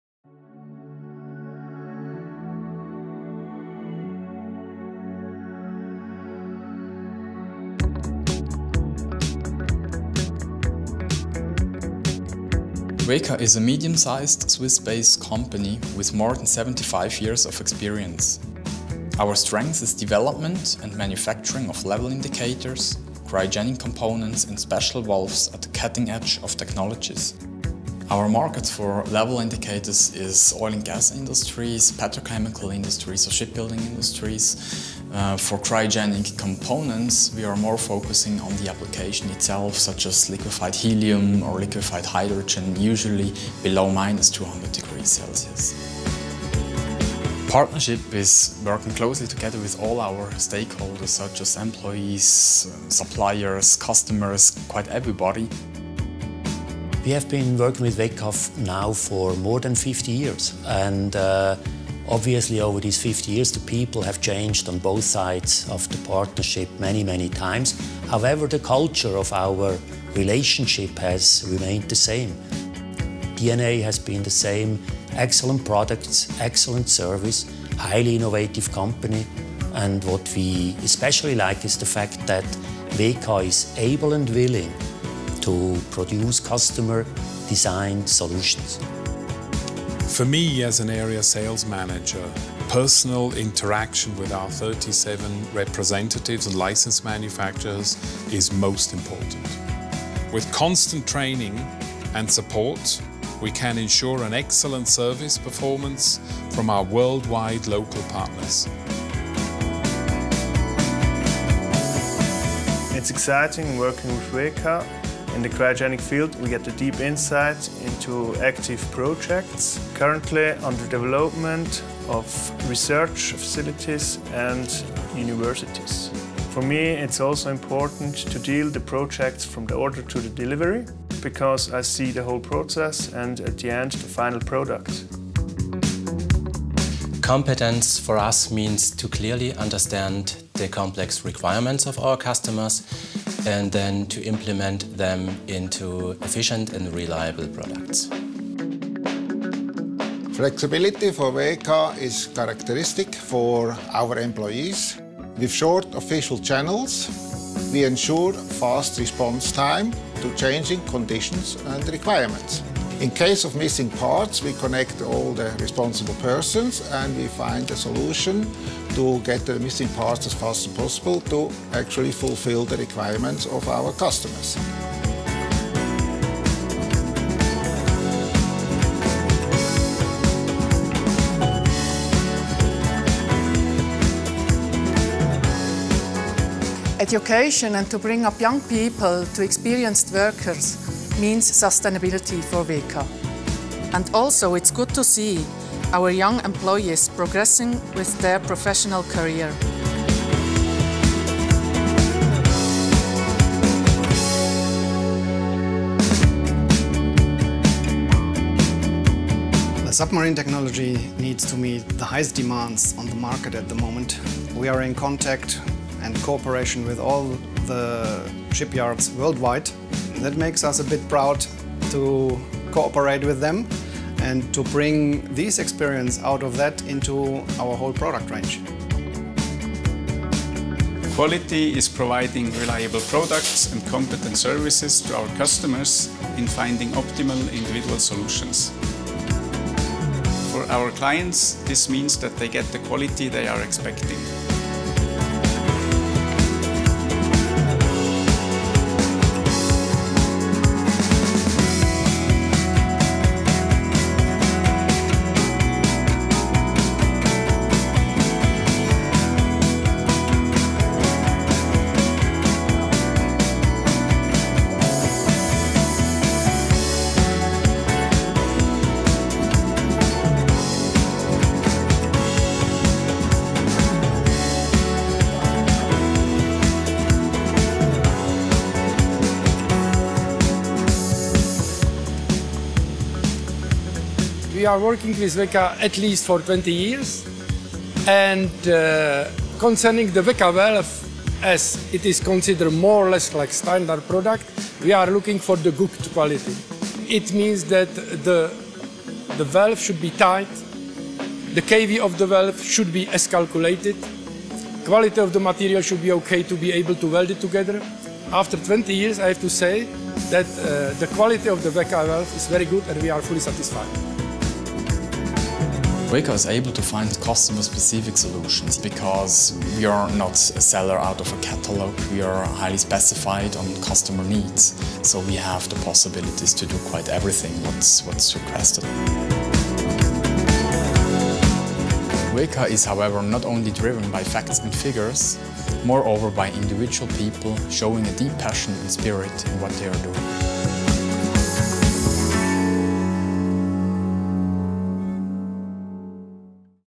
WEKA AG Imagefilm
WEKA AG in Bäretswil, Switzerland:

WEKA AG is the leading manufacturer of Cryogenic Components, Special Valves and instruments for Level Measurement. With about 60 employees the company reacts fast on client’s needs. Their strengths are development, production and testing of tailor-made solutions. WEKA AG guarantee for high-quality products and services.

More than 90 percent of the turnover is realized abroad. Sales of instruments for Level Measurement are done through a worldwide net of qualified representatives. For cryogenic components and special valves, you can address directly to us.